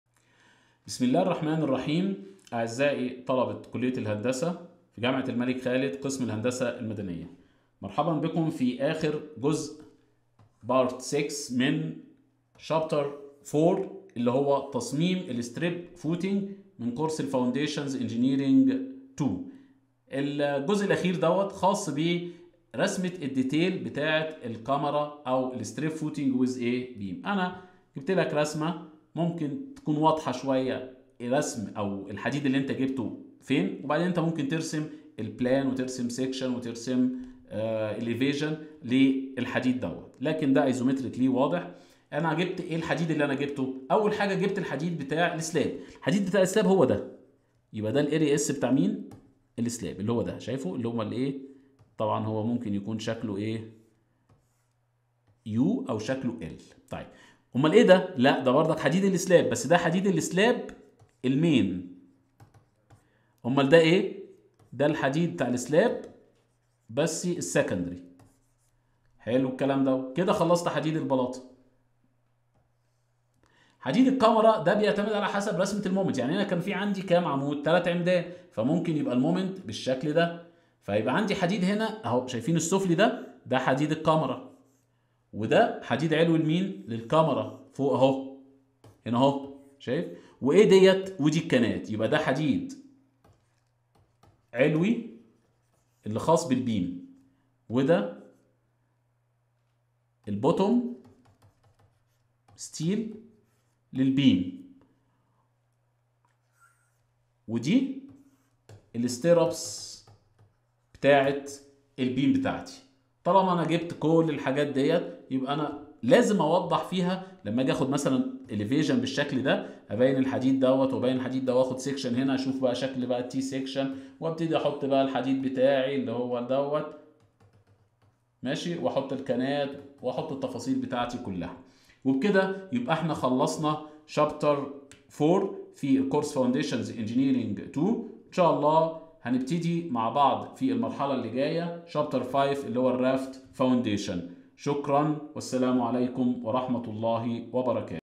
Foundations Engineering 2 | C4-L6 | Design of Strip footing with beam steps - 5
Chapter 4 : Strip footing with beam
Lecture 6 :  Design of Strip footing with beam steps - 5
Explain the Design of strip footing with beam steps (details of strip footing)